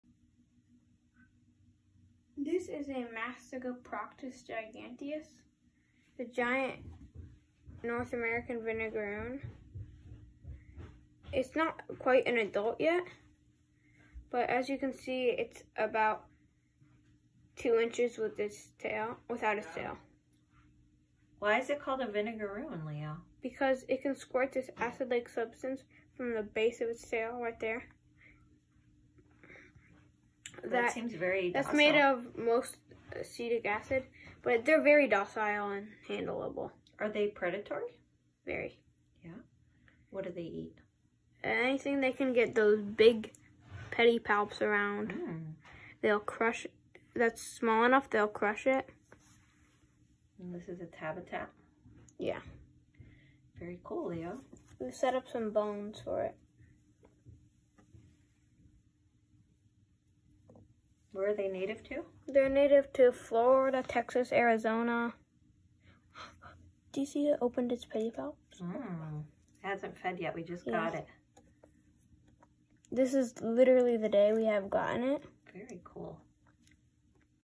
Animal Channel Episode 77: Vinegaroon!
Mastigoproctus gigantes , Leo’s pet giant vinegaroon.